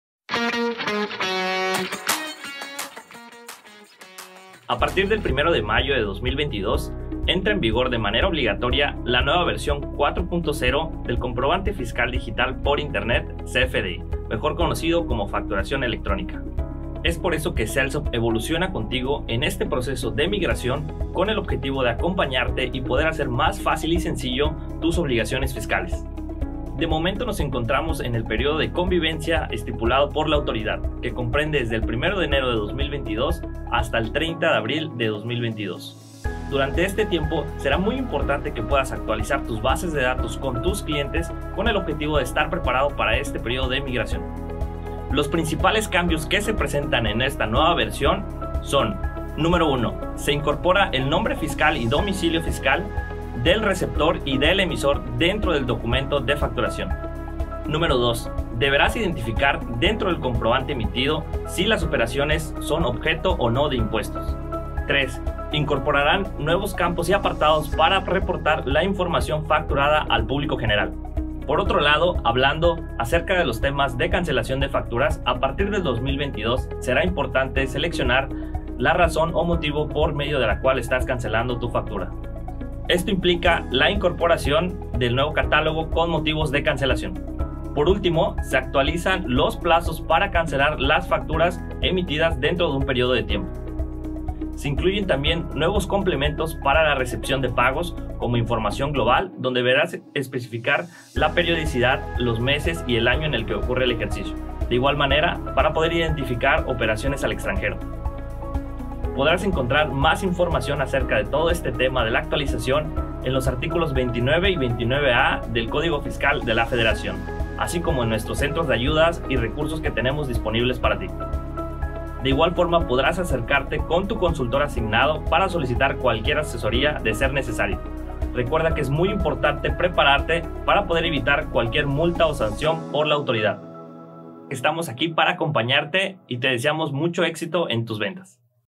FACTURACIÓN 4.0 ¿Cuáles son los cambios más IMPORTANTES?
A partir del 1 de mayo de 2022 entra en vigor la Facturación 4.0 que trae diversos cambios que buscan hacer más fácil el cumplimiento de las normas tributarias. En este video vamos a comentar los principales cambios, a fin de hacer más sencillo la transición.

📑 Índice

00:00 Inicio
00:28 Actualiza tus bases de datos
00:45  Incorporación del domicilio fiscal
00:57 Objeto de impuestos
01:05 Facturas al público general
01:11 Cancelación de facturas
01:35 Complementos de pago
02:01 Acércate a tu consultor
                                                 Controla tus ventas con SalesUp!

Videos relacionados que te pueden interesar

📞Nuestros consultores te están esperando. Pide tu demo gratis